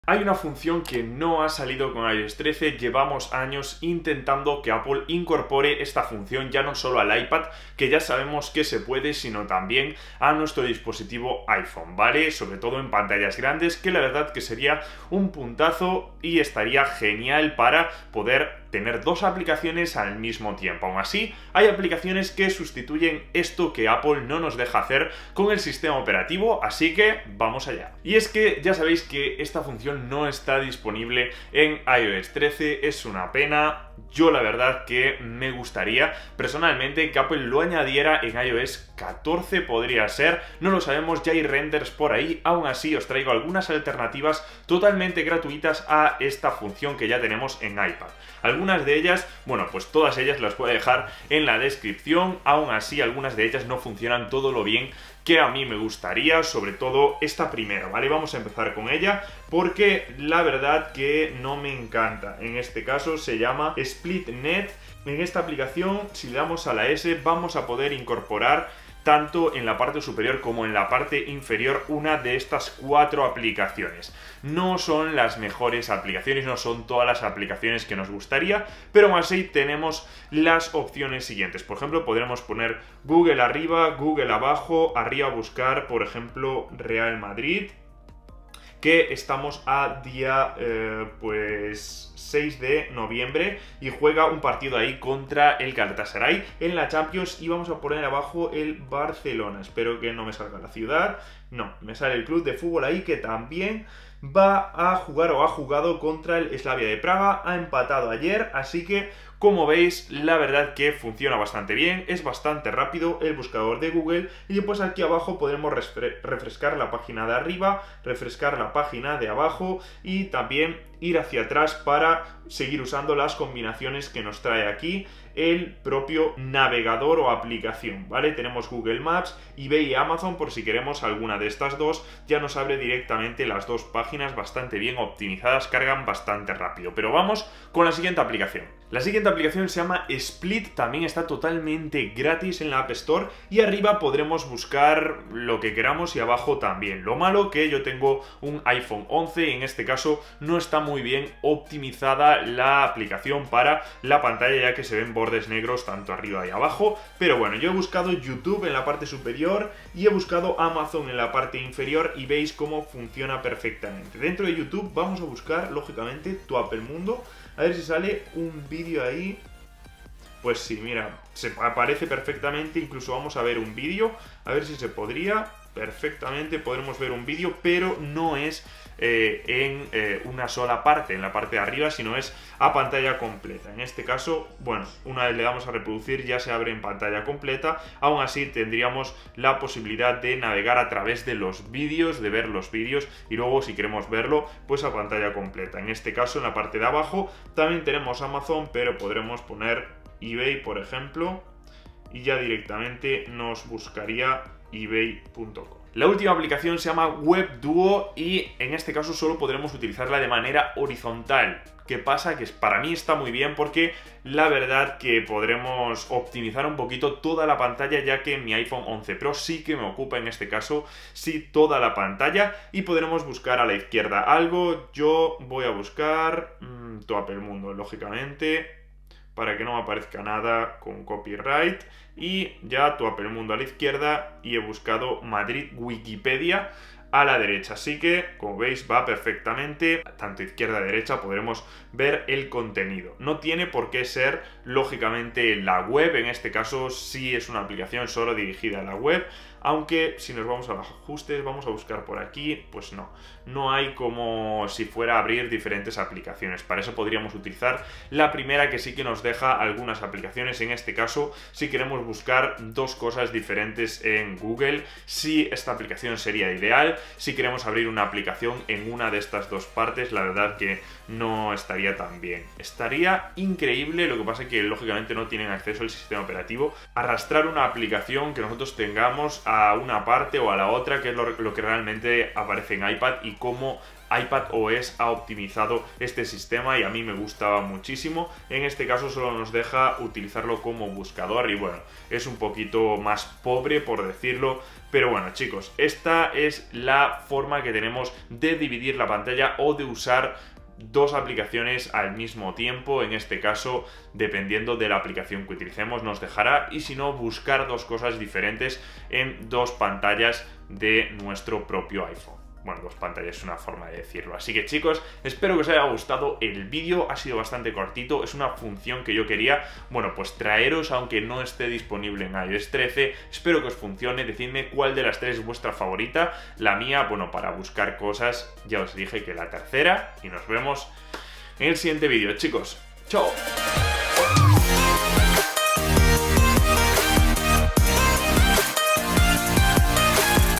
DOS APPS al MISMO tiempo en iPhone con iOS 14
Te enseño a utilizar dos aplicaciones a la vez como en iPad, con iOS 14 también se puede en iPhone. Podrás abrir una ventana multitarea y ver un video a la vez que haces compras por internet en otra, o cualquier cosa que se te ocurra.